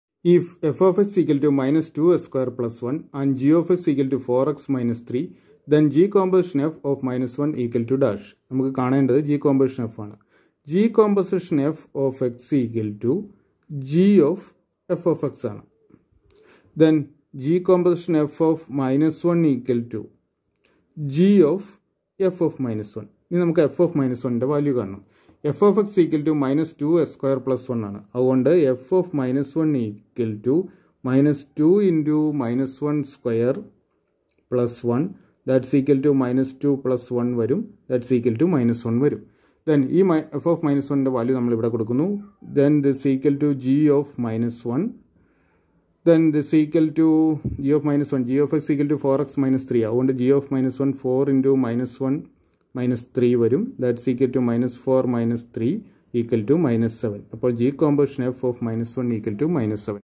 Composition of Functions / Relations and Functions/ NCERT class 12 / Plus Two Mathematics/ Malayalam
Plus Two Class 12 Mathematics
Explained in Malayalam